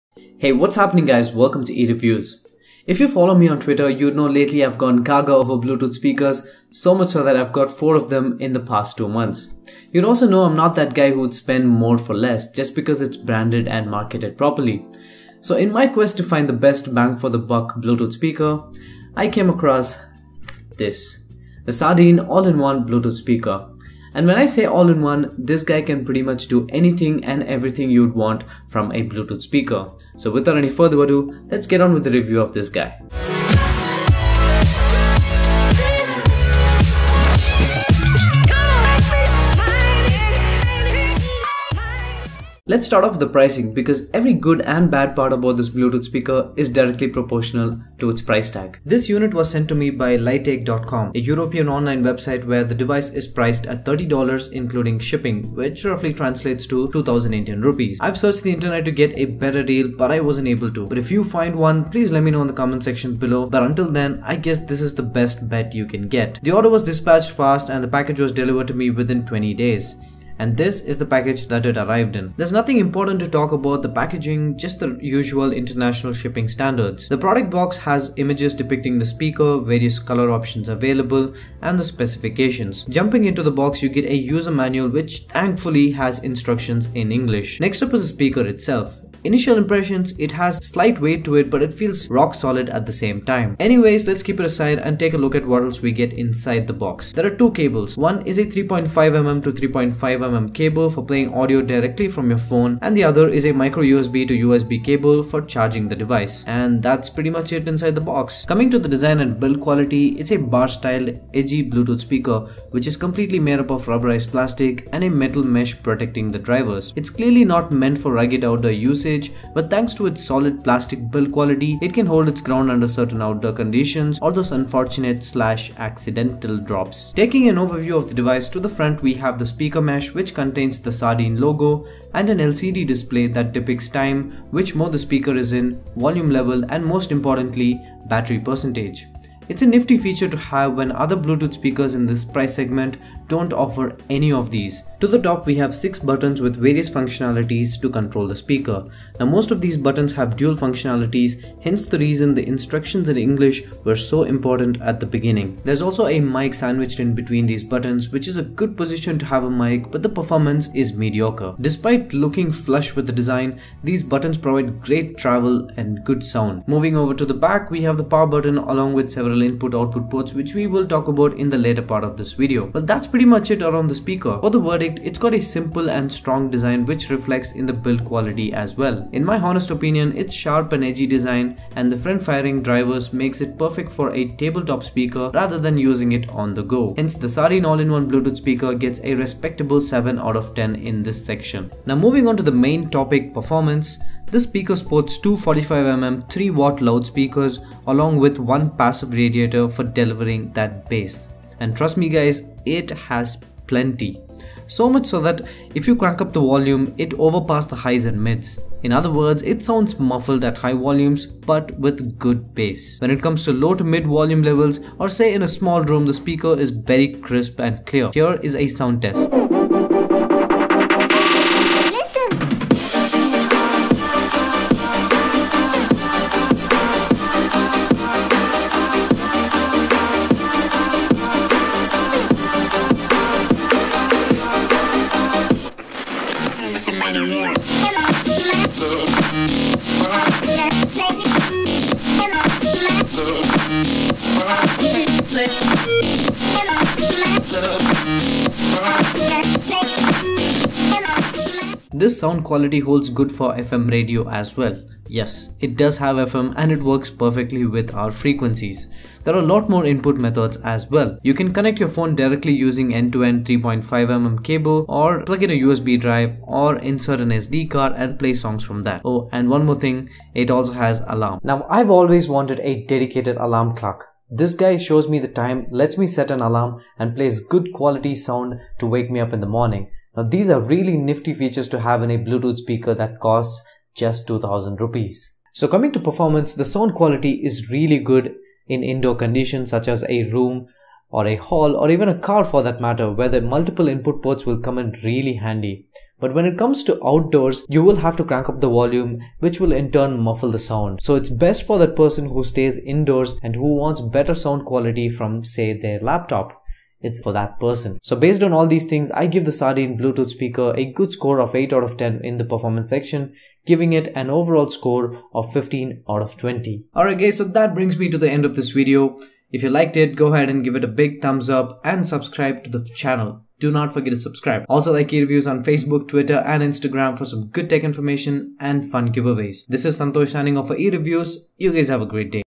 Sardine SDY-019 Review | All-In-One Bluetooth Speaker
A Bluetooth Speaker that can pretty much do anything - play music from numerous sources including FM Radio, display time, stats and lets you set an alarm!
All of the above & more for just Rs. 2000 or $30!

Bluetooth Version: Bluetooth V3.0
Wireless Transmission Range: about 15-30meters
Output/RMS: 5W X 2
Speaker: Φ45mm 3Ω; 3W X 2
SNR ≥90db (line out)
Frequency Response: 80HZ-20KHZ
Distortion: 0.3%
Battery: 3.7V 1500mAh Rechargeable Lithium Battery(Built-in)
Working Voltage: 3.3V-4.2V
Charging Voltage: DC 5V/500MA
Music format: Support MP3, WMA, WAV, APE, FLAC
Product Dimensions:240*55*61mm